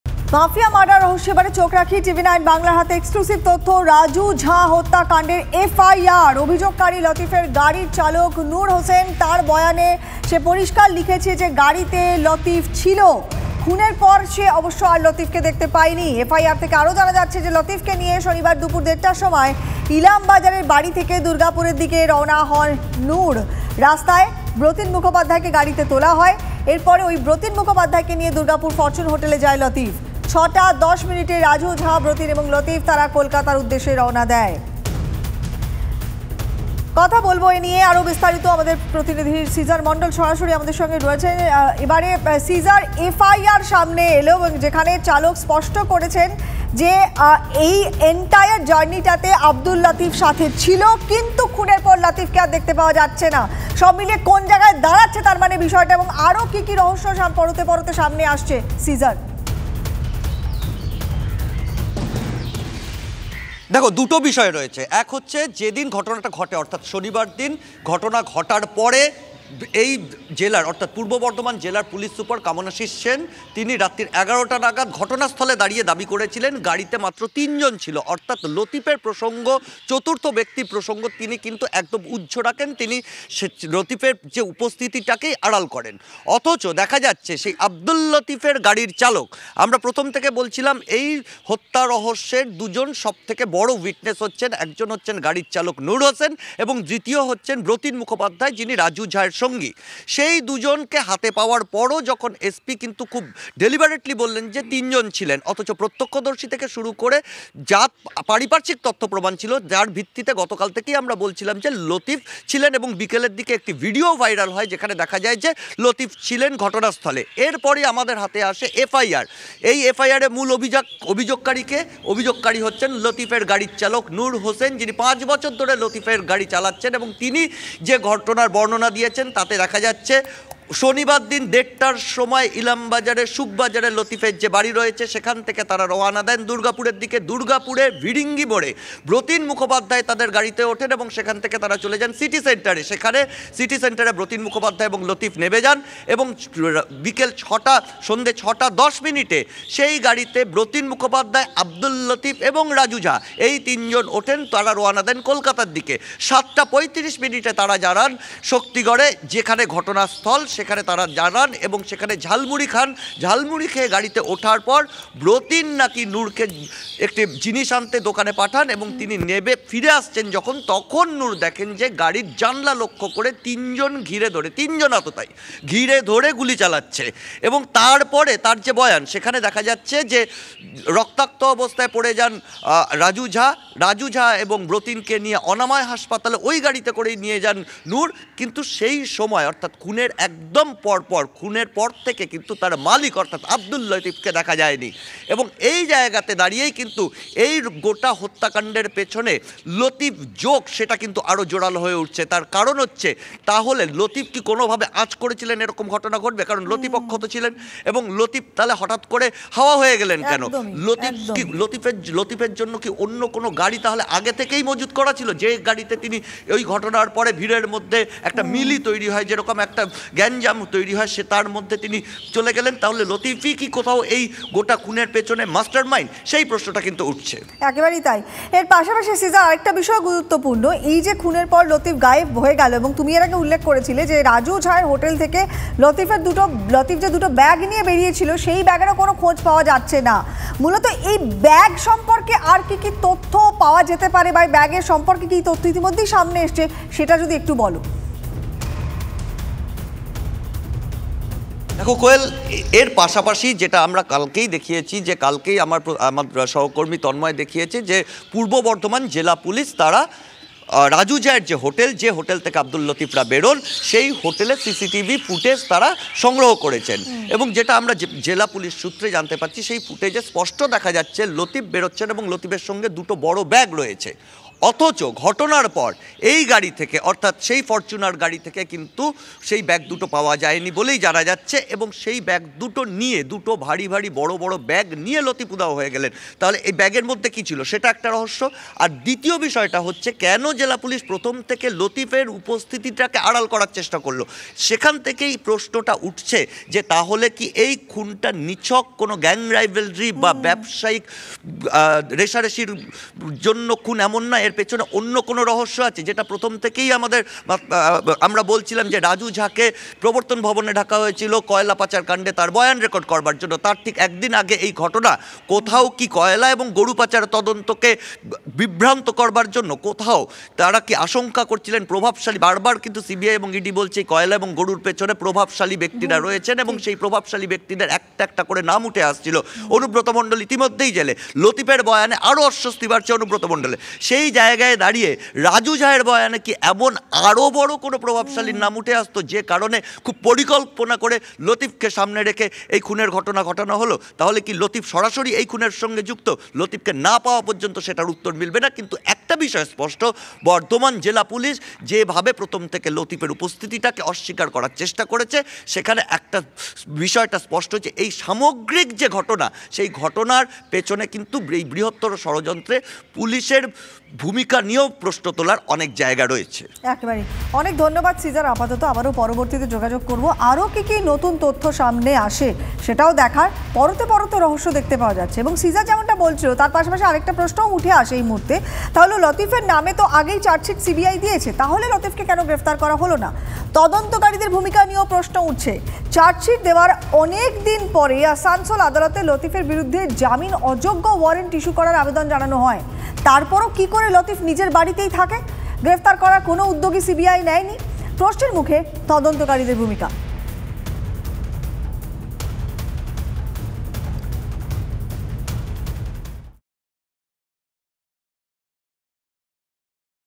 Shaktigarh Shootout: রাজু খুনের আগে গাড়িতেই ছিলেন লতিফ, TV9 বাংলার হাতে লতিফের ড্রাইভারের FIR
TV9 বাংলার হাতে লতিফের ড্রাইভার নুরের FIR। যেখানে নুর পরিষ্কার জানাচ্ছেন, রাজু খুনের সময় গাড়িতেই ছিলেন লতিফ। তবে খুনের পর তাঁকে আর দেখা যায়নি। তবে কি রাজু ঝা খুন হবেন, এমন খবর ছিল লতিফের কাছে?

Latif was in the car before Raju's murder, TV9 Bangla has an FIR of Latif's driver.